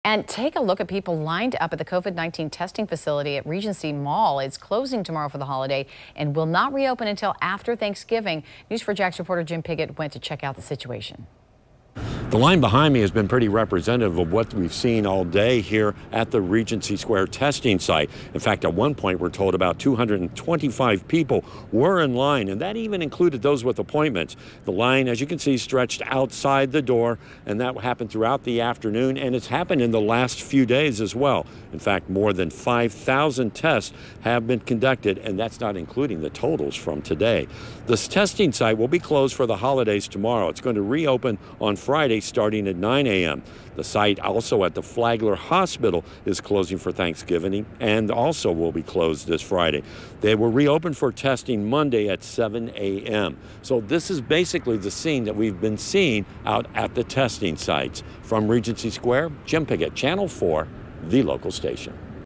COVID-19 testing sites closing for Thanksgiving
VIDEO: Hundreds of people line up at the testing site in Regency.